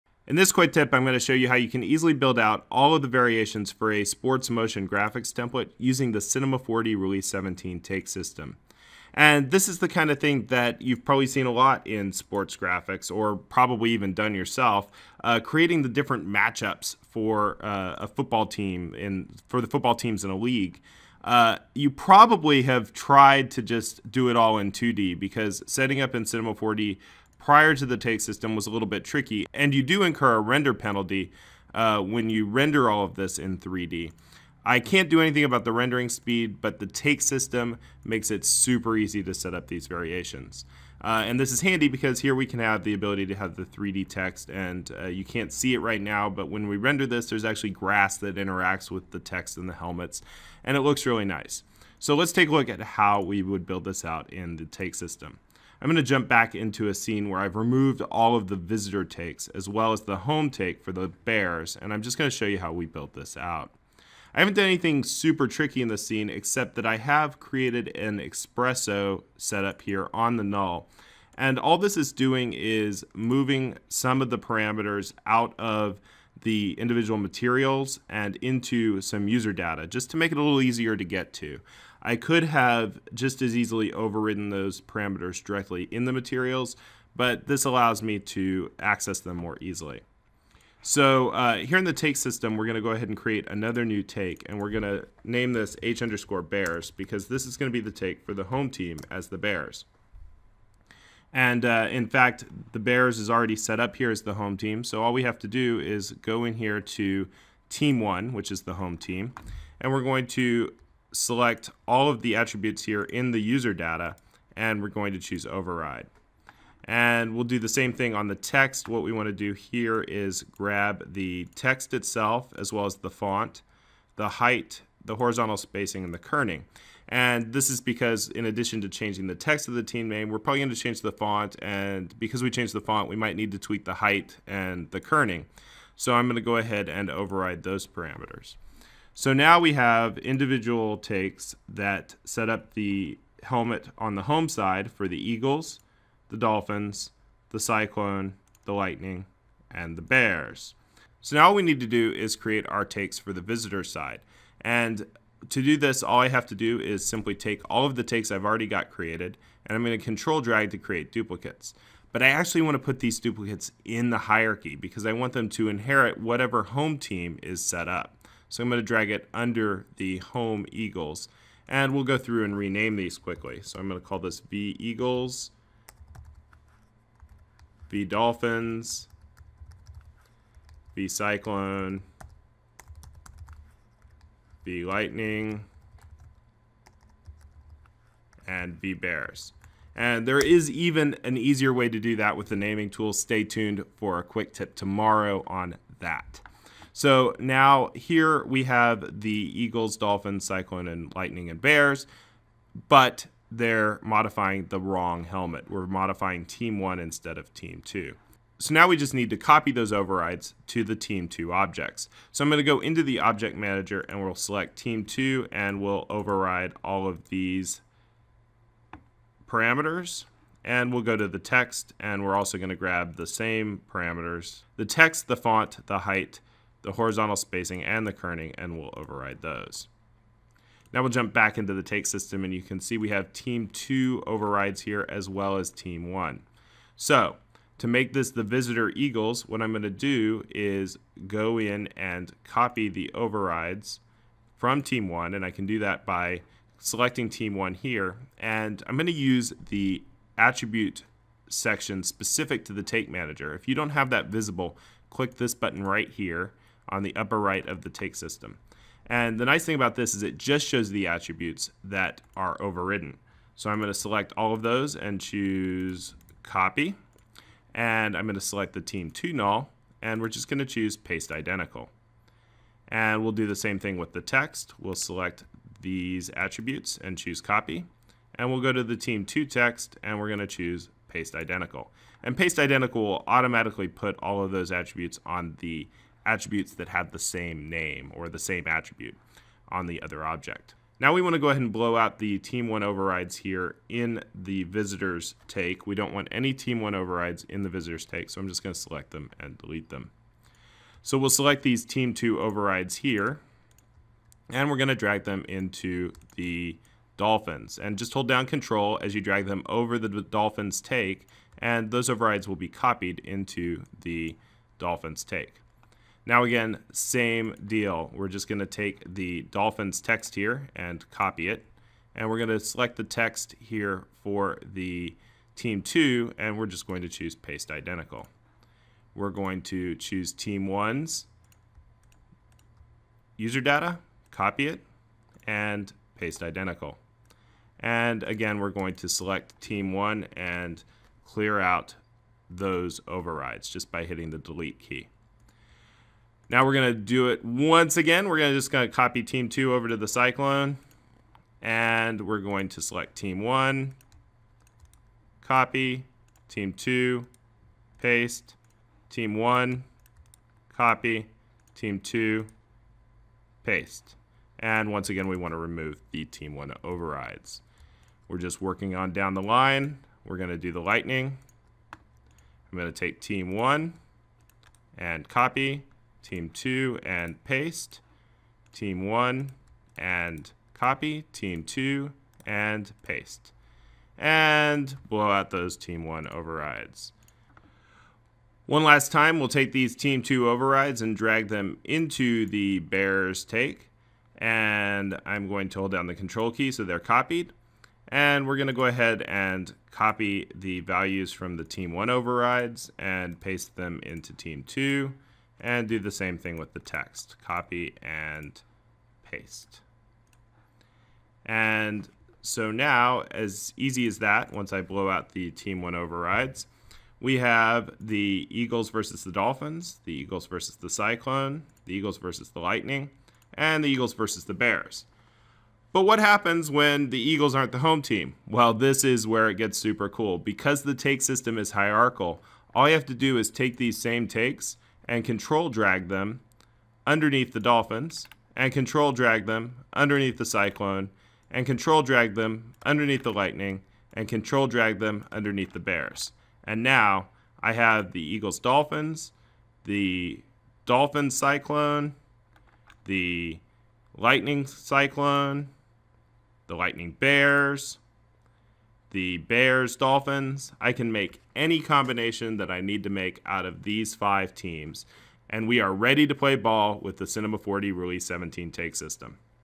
Cinema 4D Quick Tip: Create Sports Matchups Quickly with the Take System
The Take System in Cinema 4D Release 17 makes it easy to create multiple variations of a scene, like all the possible matchups between football teams in a league. In this tutorial, you'll see how you can quickly build variations by copying overrides, modifying attributes in the Take Attribute Section, and taking advantage of Take hierarchy.